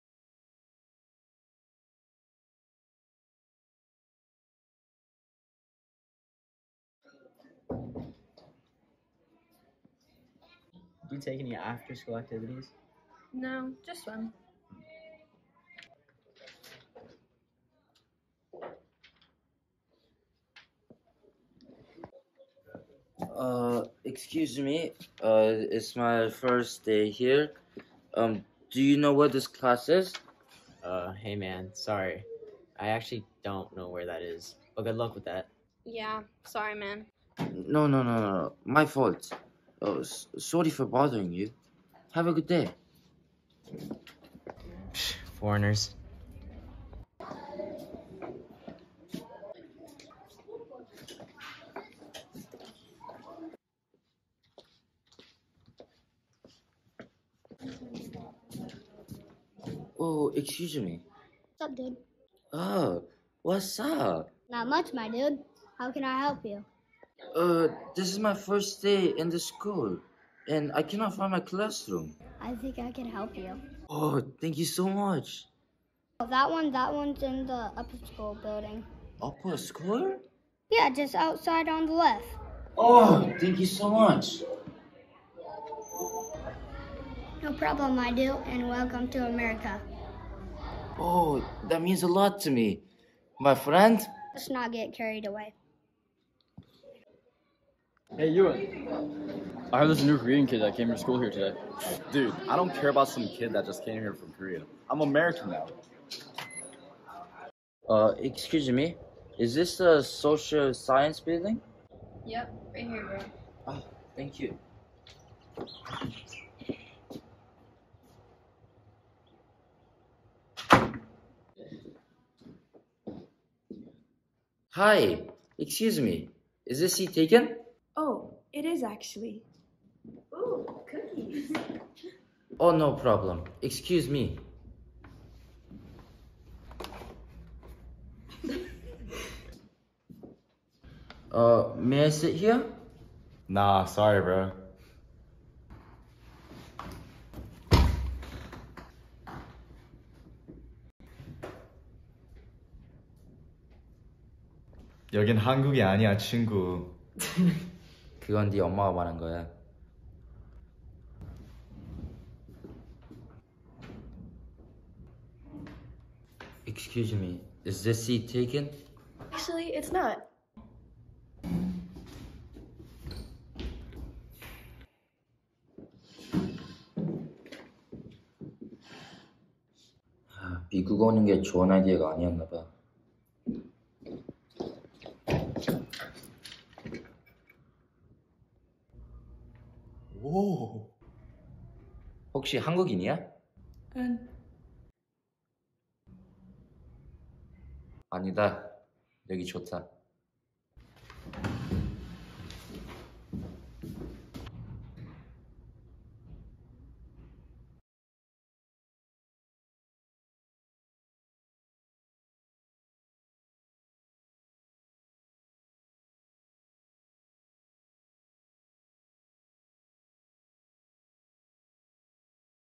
The Foreigner
A foreign exchange student struggles to find his first class and place at his new school. The other kids are anything but welcoming, but he discovers both compassion and racism from the most unlikely of sources. A high school movie written by Korean born professional actor, singer, emcee, dancer, and Jr Olympic swimmer Eli Lee, the short film follows the 1st day at school for a new student from Korean trying to find his place at his new American school. Our students write, direct, produce, and edit their own short films to develop their acting and film making skills. Come join the crew today! Ig: @hamastudios Fb: Hawaii Acting & Modeling Academy